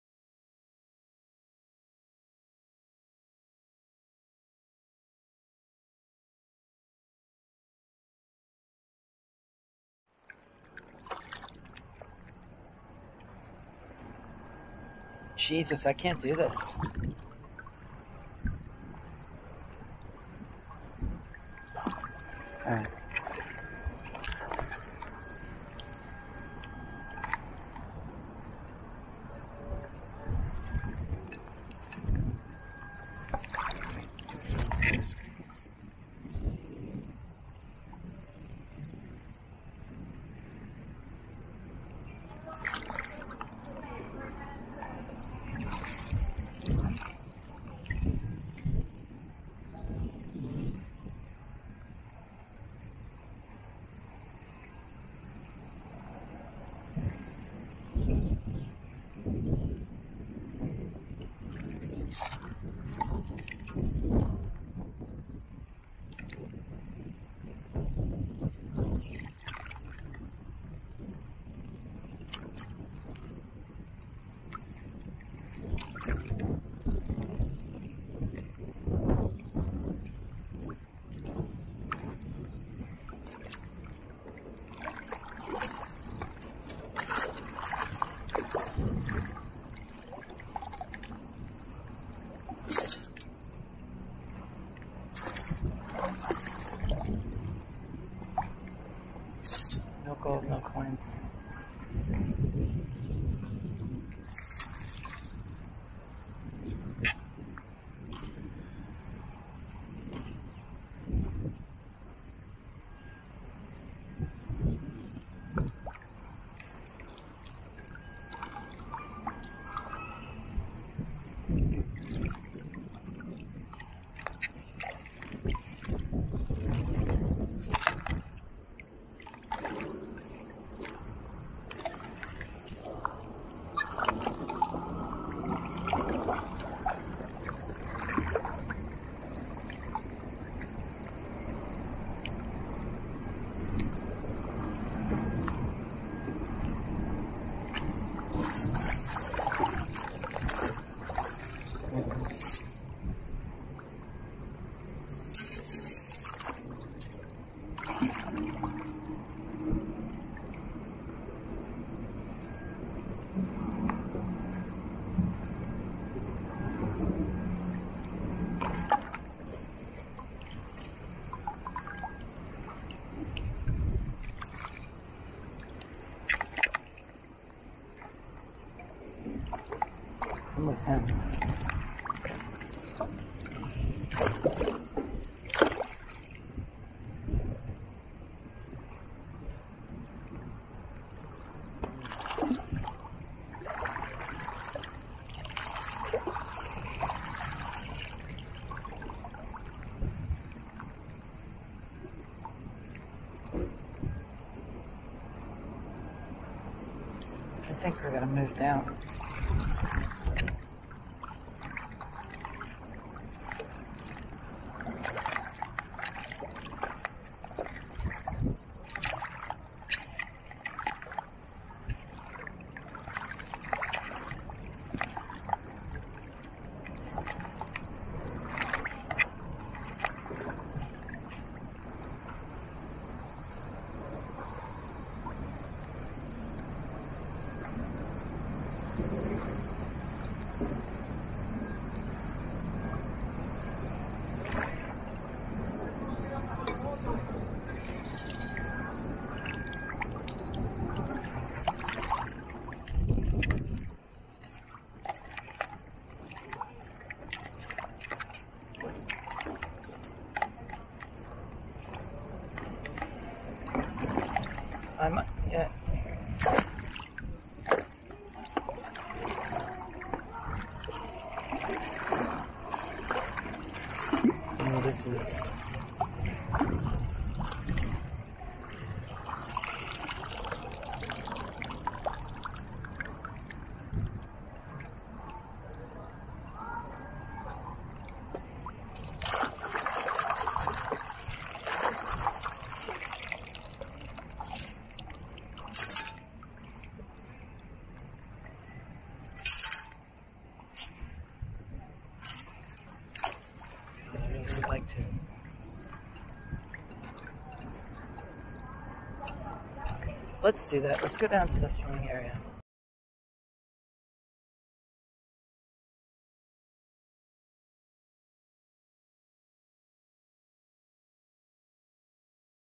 Palm Beach - Phil Foster Park - Metal Detecting in a Cloud of MUD
Watch the chaos of trying to recover targets in a bay lined with mud and thick with shells.  Metal detecting struggles.  Never quit, never give up.